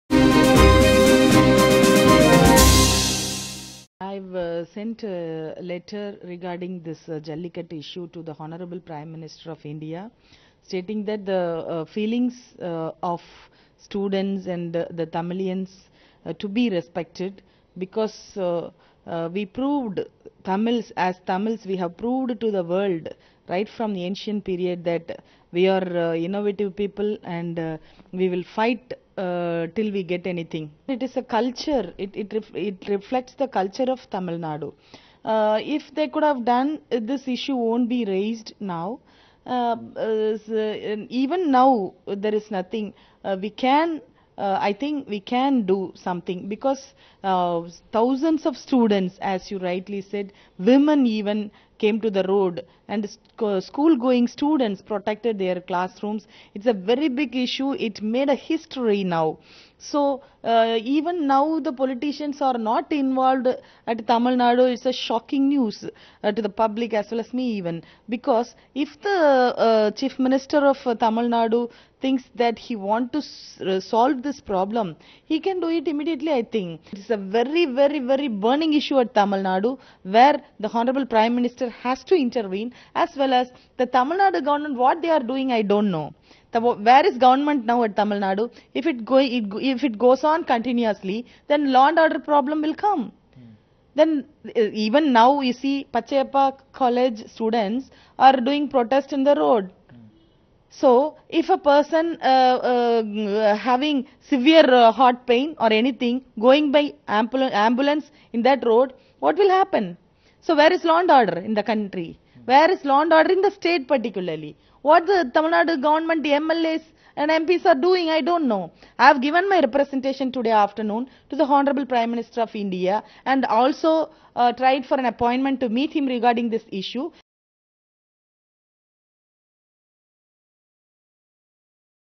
Sasikala Pushpa urges PM Modi to interfere into Jallikattu matter | Oneindia News
Expelled leader of the All India Dravida Munnetra Kazhagam (AIADMK) Sasikala Pushpa has written a letter to Prime Minister Narendra Modi urging him to interfere in the issue of jallikattu. She asserted that the bull-taming sport is linked to the emotions of Tamilians, Pushpa said that the Prime Minister must come up with a solution on the matter. She also questioned the working of the present state Government, saying that law and order problems will follow if the issue is not resolved.